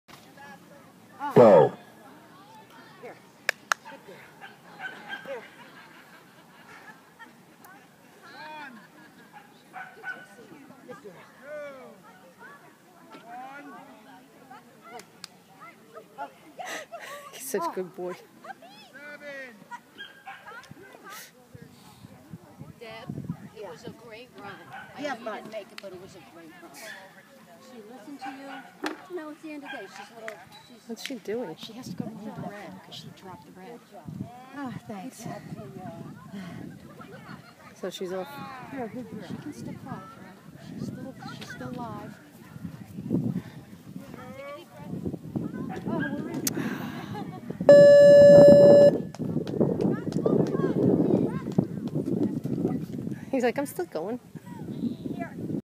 Rascal's Snooker run from CPE
This is Rascal's Snooker run in CPE on September 7, 2013. He did so good! He didnt Q but he was still amazing!